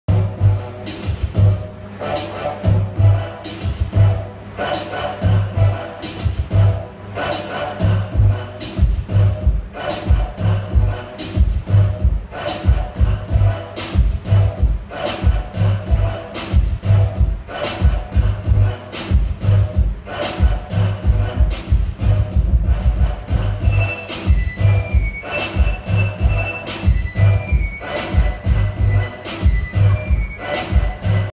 Tarraxinha warm up with Teejay
Teejay from United Motion Dance warm up for Kiz plus Beatz class in Melbourne, Australia.